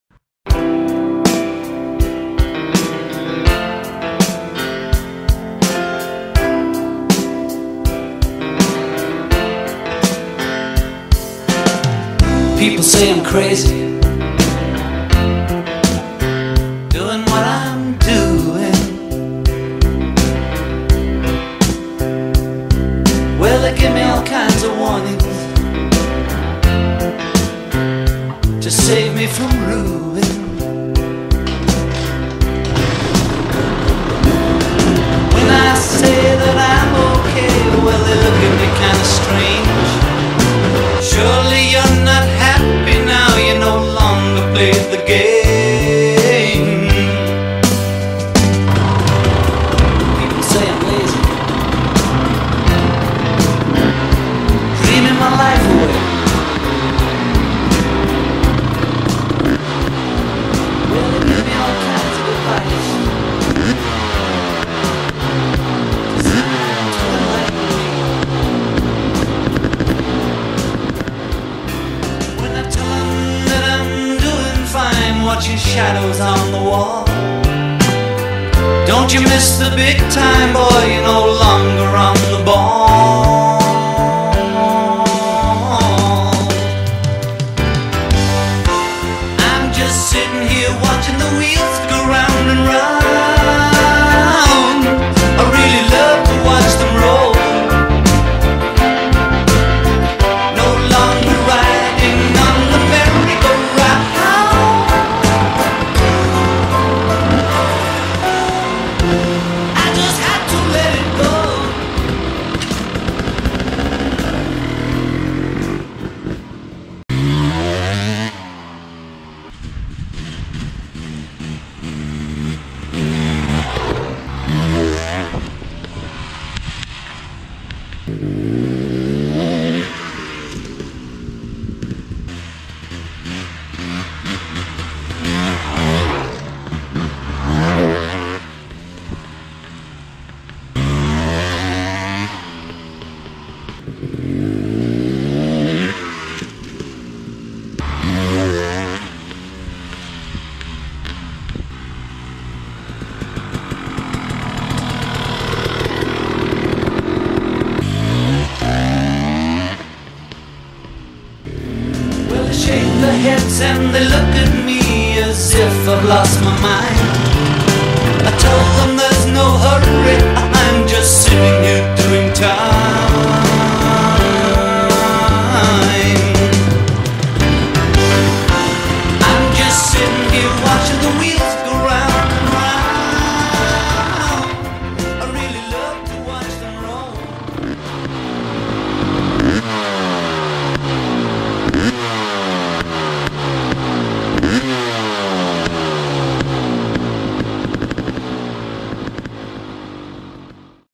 First Start Up w/ Lectron Carb | KTM 250sx
Installed a Lectron H series carb on the '15 250sx. Haven't gotten to truly test the carb yet but here's a short video of the initial startup...
Didn't film any of the installation process but it was  quick & easy to install. So far it feels responsive with a good, strong pull off the bottom...will know more after a proper ride
Bike: 2015 KTM 250sx
Camera: GoPro Hero 8 Black